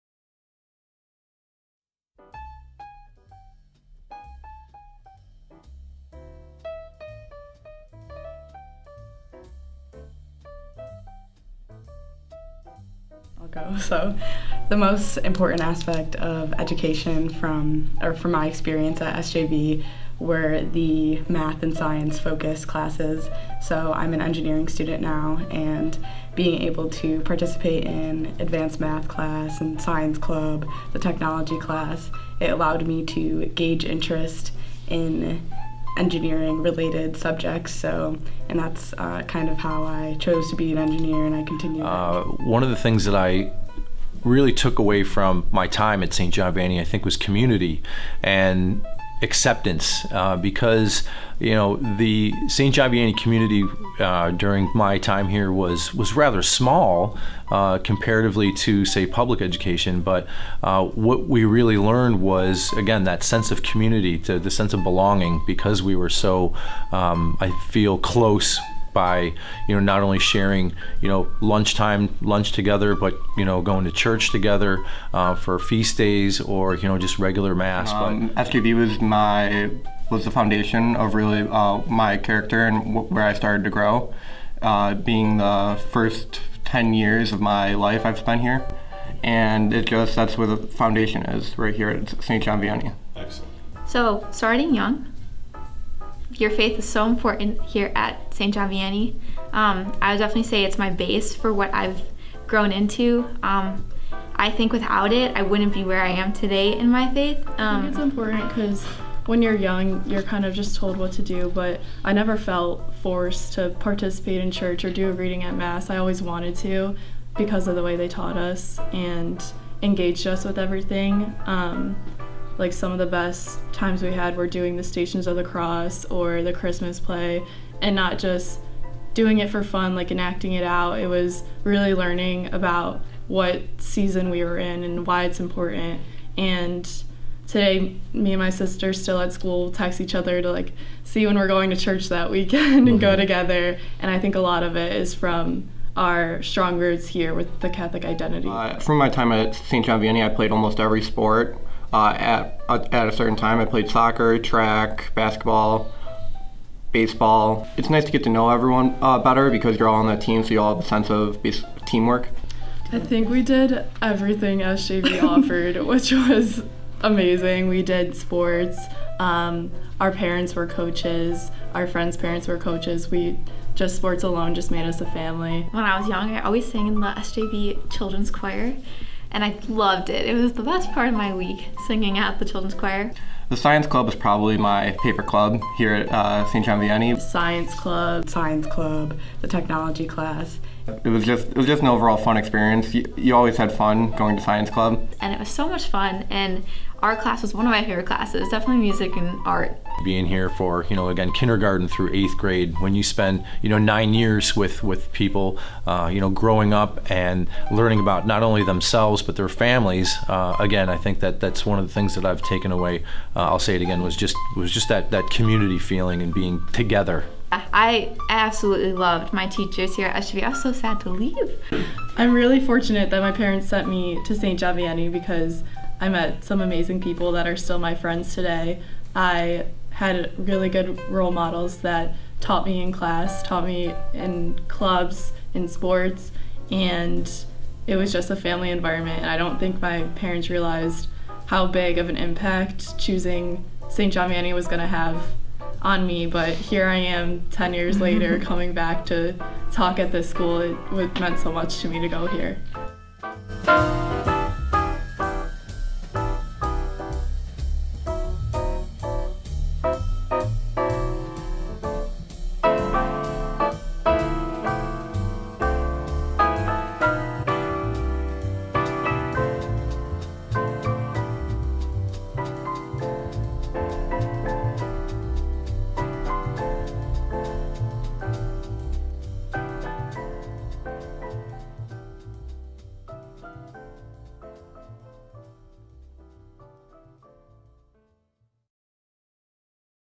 SJV SCHOOL PROMOTIONAL VIDEO
Former students from Saint John Vianney School in Orchard Park, New York tell us about their experience.